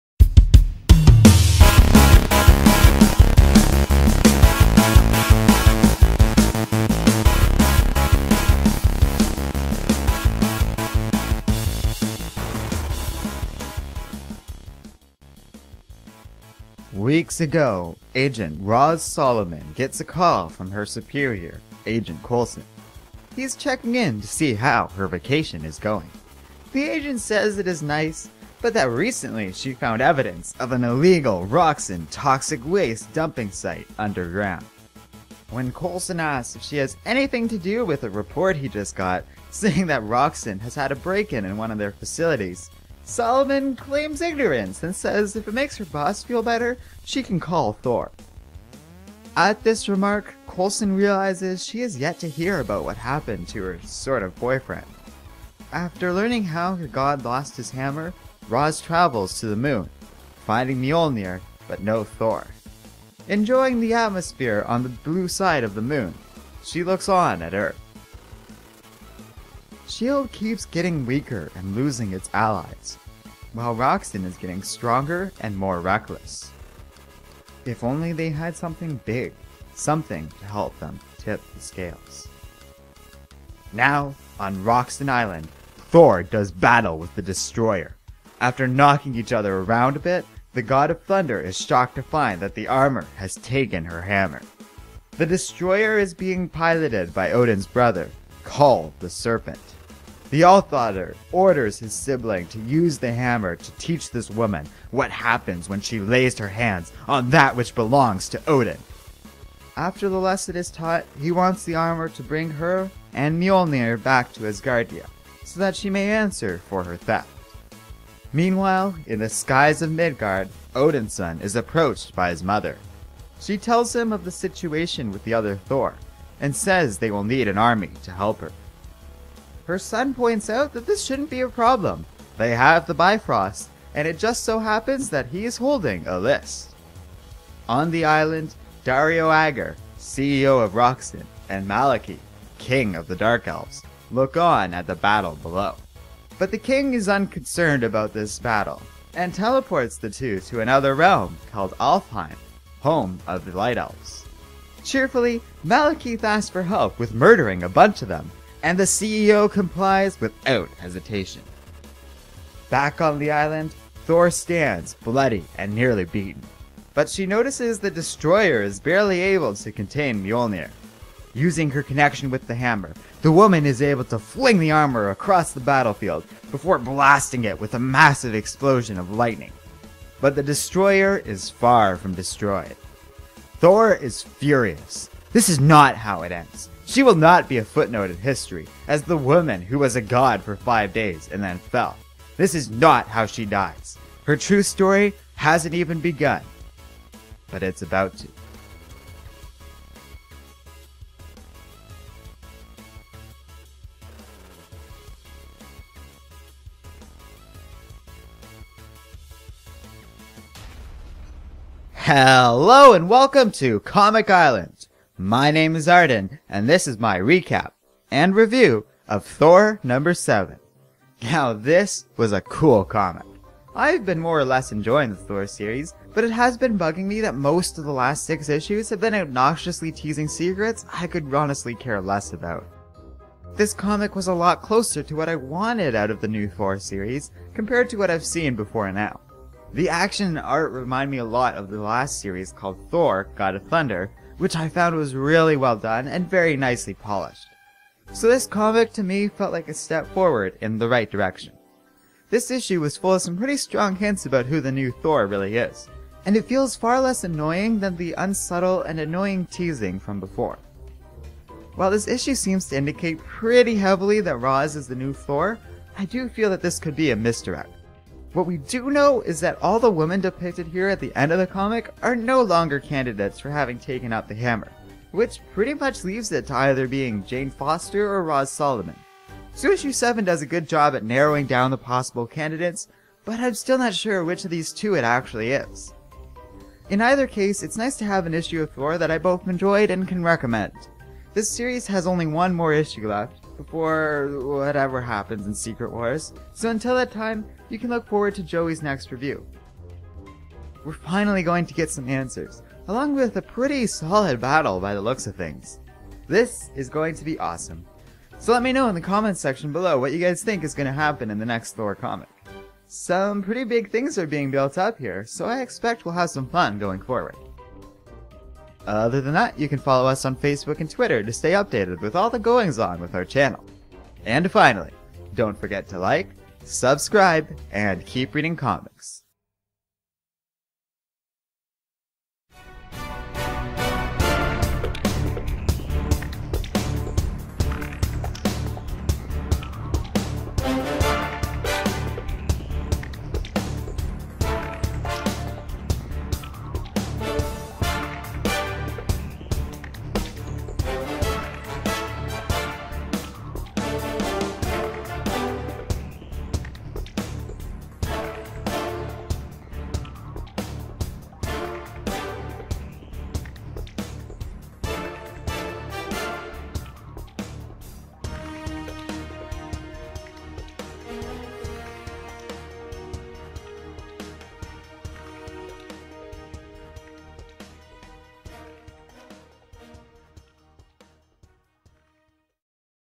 Thor #7 Recap/Review: The Battle for the Hammer - COMICS MADE BY KIDS
Arden recaps and reviews the latest issue of Thor. An old enemy returns, and he's not here for hugs.

Origins/Bio - /

Comic Events - /

Solicitation info:

Thor #7

by Jason Aaron (Author), Russell Dauterman (Art), and Marvel Comics (Publisher)

THE BATTLE FOR THE HAMMER RAGES ON! BEHOLD, THE ALL-NEW THOR VS. THE UNSTOPPABLE MACHINE OF DEATH AND DESTRUCTION THAT IS... THE DESTROYER. MOST VERILY, 'TIS 'NUFF SAID.

Cover Price: $3.99; Rated T